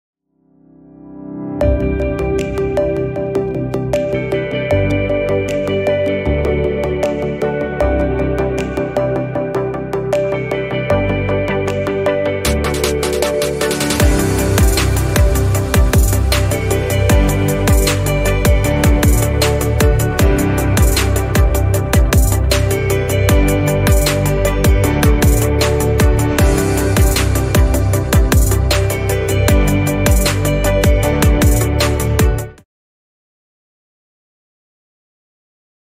Can You Identify the Best Sheep Herding Breed? Take the Quiz Now!
Do you think you know all about sheep herding dogs? click here and take this quiz to find out. and get a small facts as well.